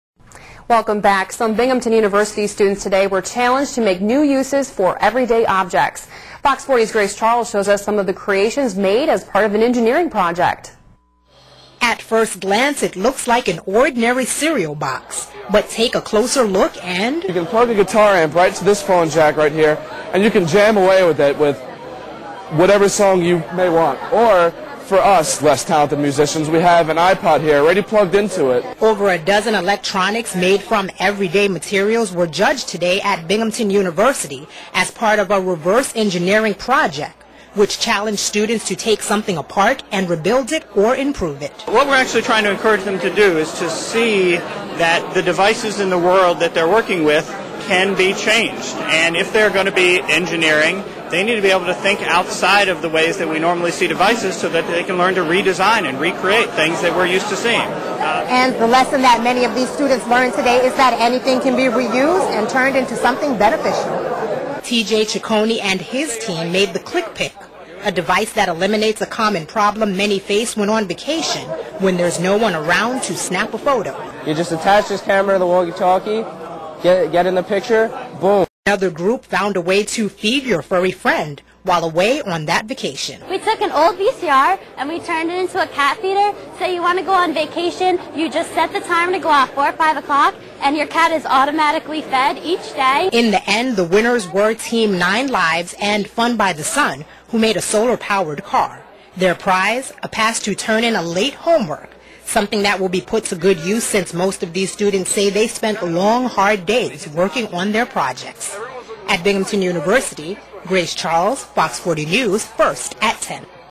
Reverse Engineering by Students in the Watson School at Binghamton
At first glance it looks like an ordinary cereal box, but take a closer look and: "You can plug a guitar amp right into this phone jack right here and you can jam away right here with whatever song you may want. For us less-talented musicians we have an iPod right here already plugged into it," said Greg Asmus of Team Sound of Breakfast.

Over a dozen electronics made from everyday materials were judged today at Binghamton University as part of a Reverse Engineering Project which challenged students to take something apart and rebuild it or improve it.

"What we're actually trying to do is encourage them to see that the devices in the world that they're working with can be changed and if they're going to be engineering they're going to have to be able to think outside of the ways that we see normal devices so that that can recreate and redesign things we're used to seeing," said Matt Laferty, a lecturer in Engineering.

The lesson that many of these students learned is that anything can be reused and turned into something beneficial.

T.J. Ciccone and his team made the Click Pic, a device that eliminates a common problem many face when on vacation when there's no one around to snap a photo.

"You just attach this camera to the walkie talkie, get in the picture boom, press the walkie talkie and you have a picture," said TJ Ciccone of Team Click Pic.

Another group found a way to feed your furry friend while away on vacation.

"We took an old VCR and we turned it into a cat feeder, so you want to go on vacation, you just set the timer to go off four five o'clock and your cat is fed automatically each day," said Wende Wells of Team 9 Lives. In the end the winners were Team 9 Lives and Fun by the Sun, who made a solar-powered car.

Their prize -- a pass to turn in a late homework assignment, something that will be put to good use since most of these students say they spent long, hard days working on their projects.

FOX 40'S Grace Charles reporting***